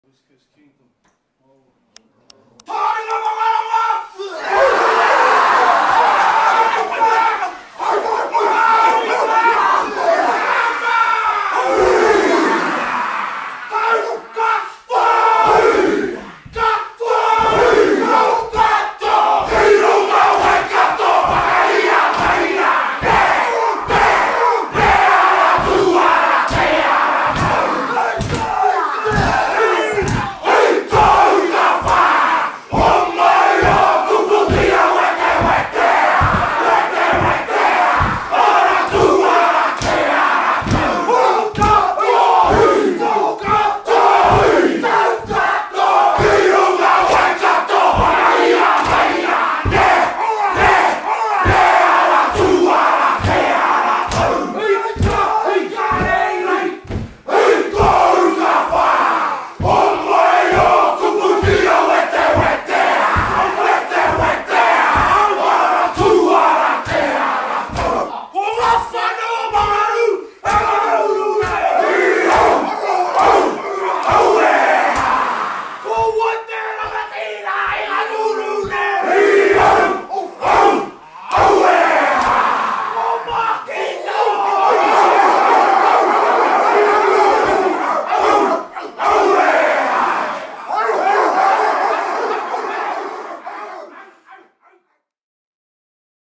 Mongrel Mob haka at Amend Taranaki wananga
Amend Movement get the highest show of respect with this powerful haka from the Mongrel Mob West Coast Chapter.